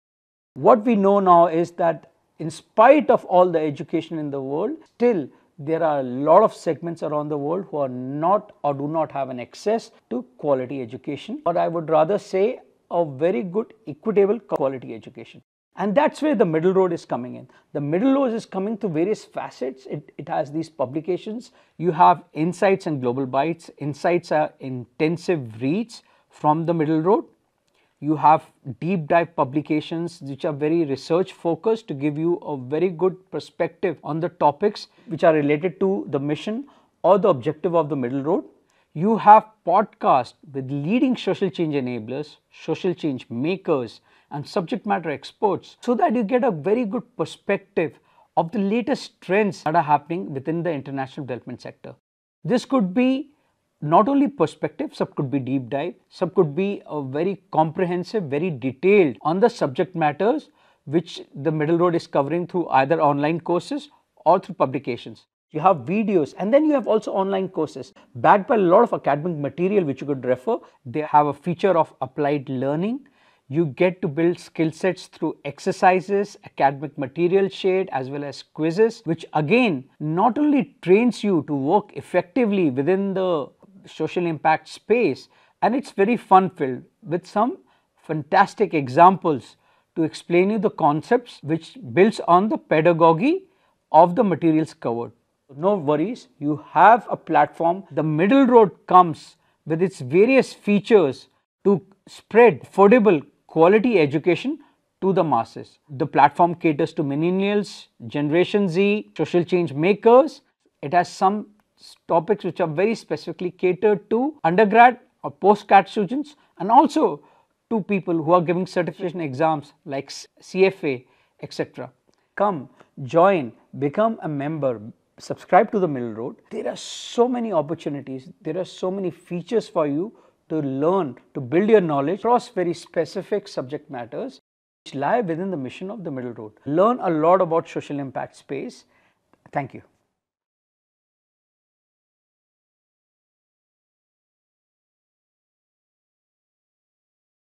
Nishant Malhotra on The middle Road | Introduction
Nishant Malhotra, Sole Founder, The middle Road platform | Middle Road OPC Pvt Ltd, talks about the mission of the thought leadership startup. Listen to an enlightening conversation on the holistic mission of the fantastic social impact company.